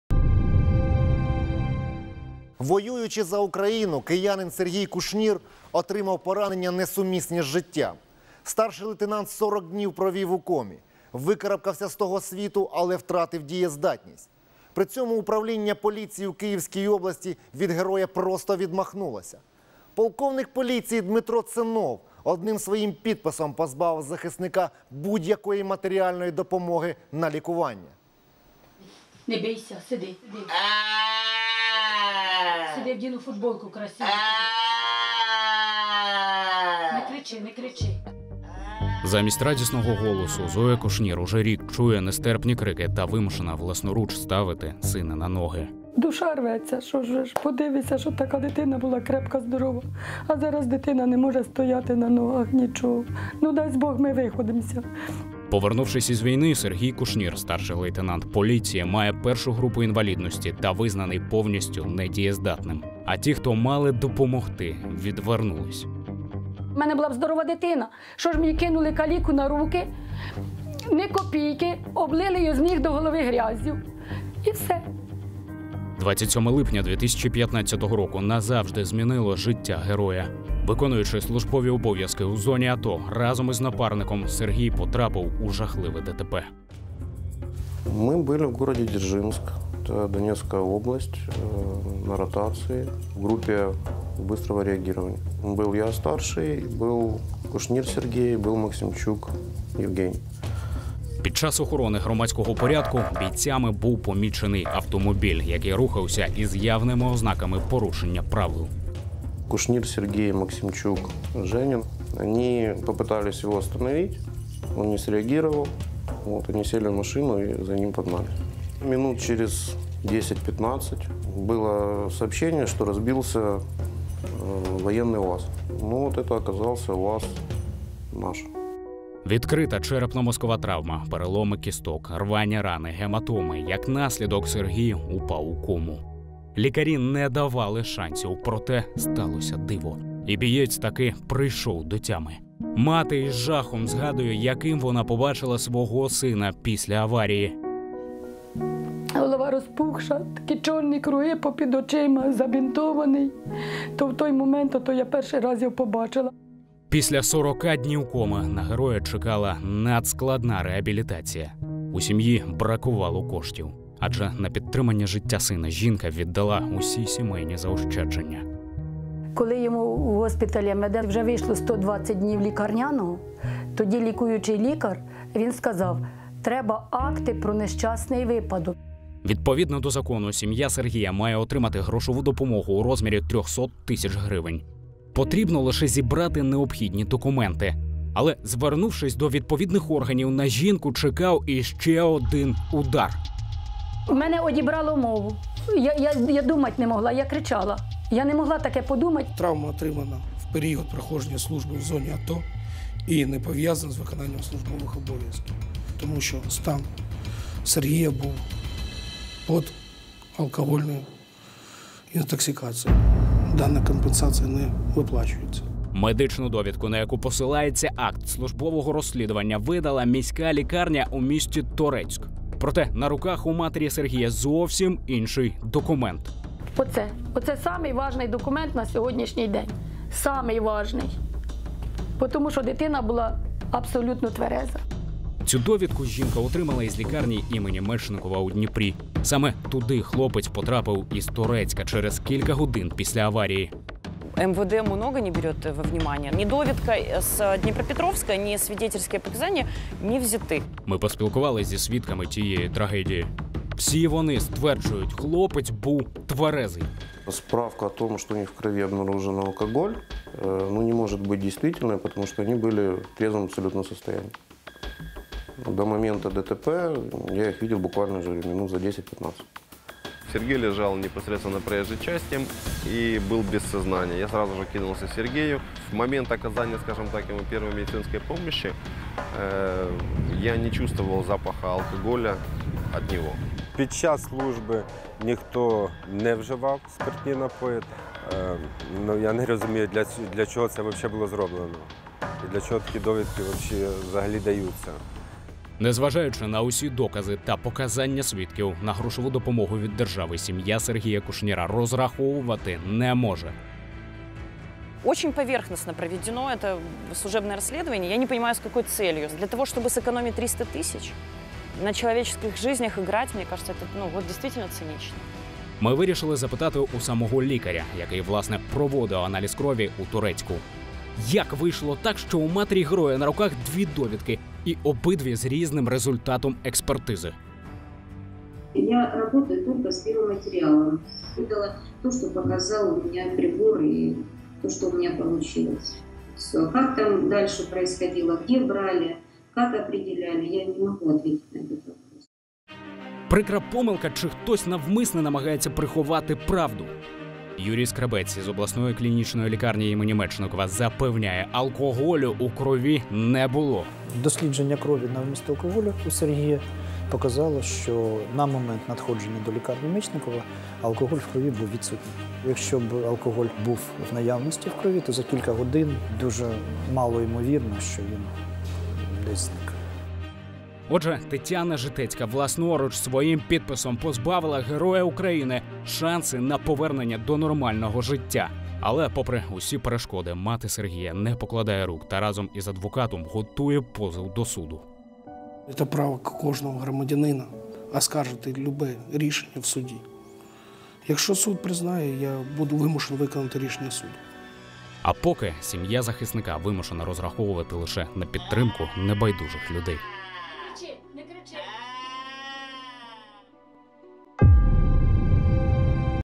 Сергій Кушнір викарабкався з того світ але досі не отримав матеріальної допомоги на лікування
Воюючи за Україну, киянин Сергій Кушнір отримав поранення несумісні з життям. Старший лейтенант 40 днів провів у комі, викарабкався з того світу, але втратив дієздатність. При цьому Управління Поліції у Київській області від героя просто відмахнулося.